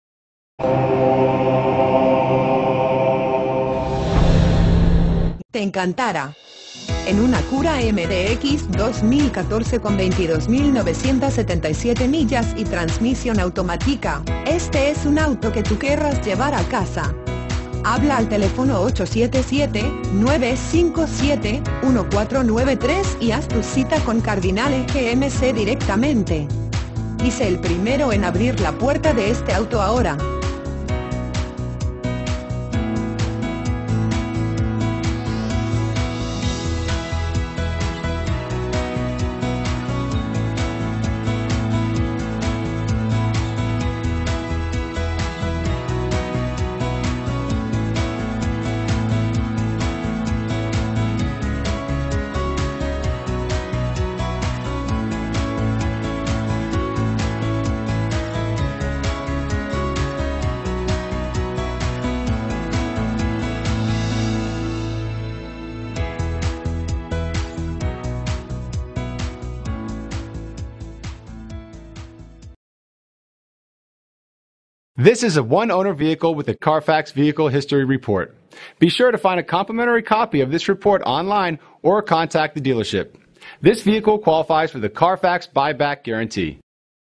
2014 Acura MDX Seaside CA U26067
Cardinale GMC
2 Heitzinger Plaza

RARE HARD TO FIND. HURRY!!!, CLEAN CARFAX, ONE OWNER, SUPER LOW MILES!!!, RED TAG SALE!! HURRY. , LEASED VEHICLE, SERVICED REGULARY, MP3 PLAYER, and LEATHER. Power moonroof. Confused about which do-it-all vehicle to buy? Well look no further than this superb-looking and fun 2014 Acura MDX. Such a reliable SUV, with low, low mileage, does not come up for grabs very often, so you better act fast. Vehicle price and availability are subject to change without notice.